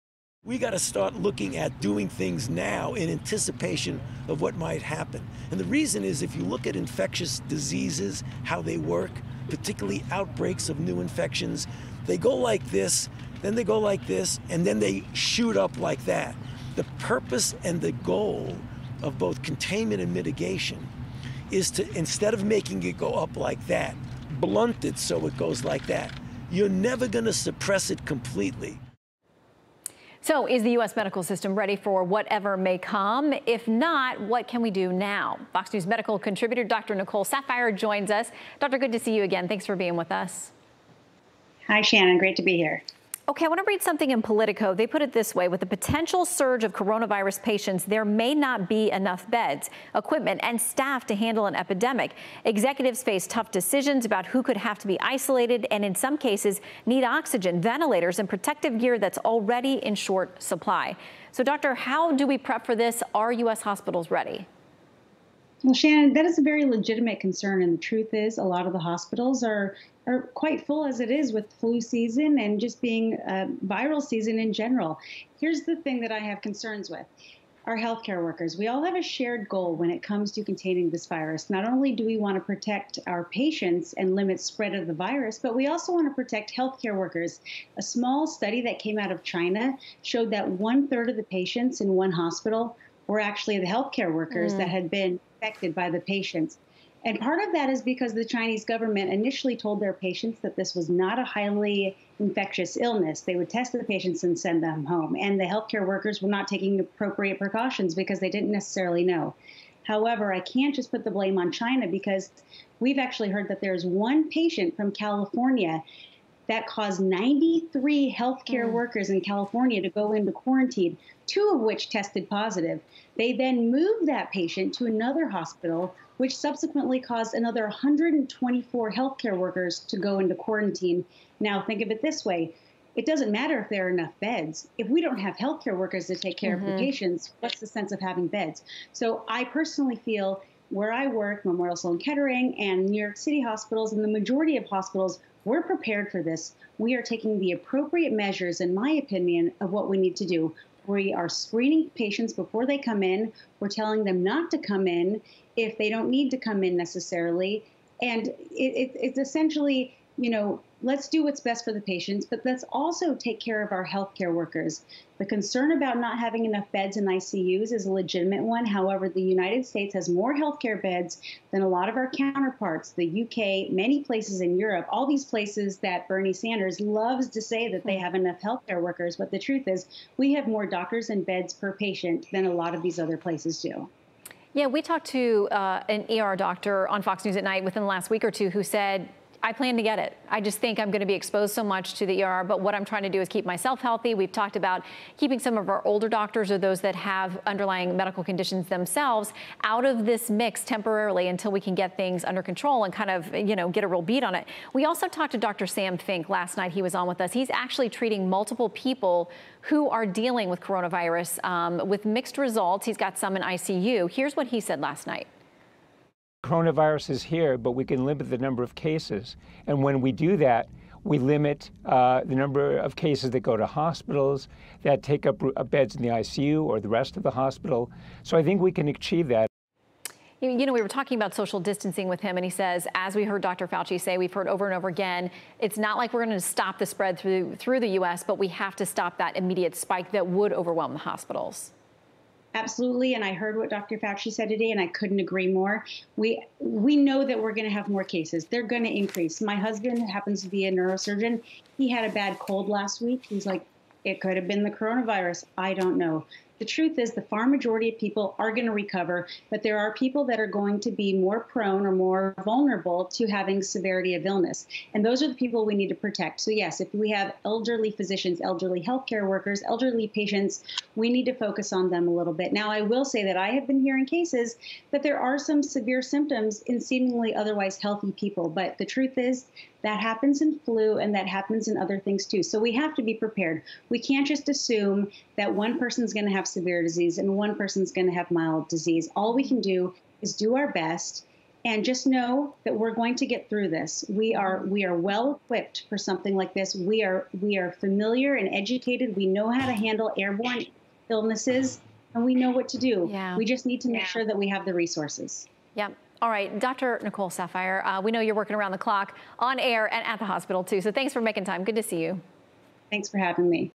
US hospitals brace for potential strain from coronavirus
How are hospitals preparing? Fox News contributor Dr. Nicole Saphier weighs in.